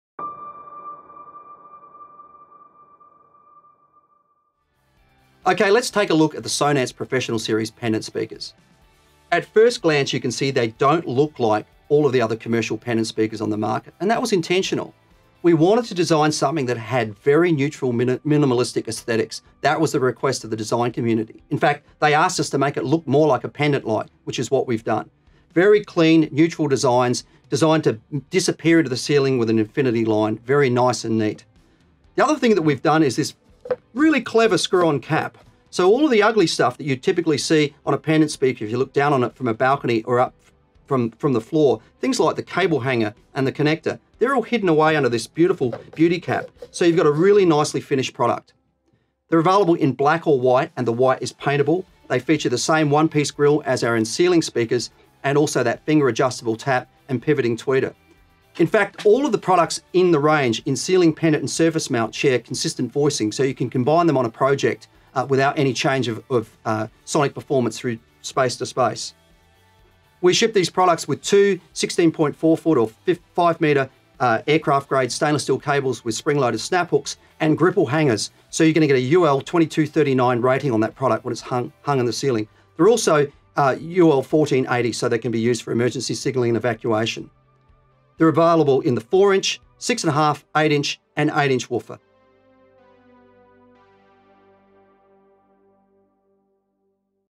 Sonance | 2min Tech: Professional Series Pendant Speakers
Sonance Professional Series Pendant Speakers utilize a minimalistic design similar to pendant lighting to blend discreetly into the environment.

Sonance’s Laminated Core Transformer combined with state-of the-art driver design delivers best-in-class fidelity and uncompromised sonic performance in 70V/100V.

Available in black or white (paintable)
Heavy-duty voice coils for reliability and longevity Magnetic grille retention for easy installation
Pivoting 1” soft dome chambered tweeter to direct sound for accurate coverage.

Front-mounted tap and bypass selector for easy adjustment
. High excursion Polypropylene woofer for effortless bass response.

One-piece bezel-less grille.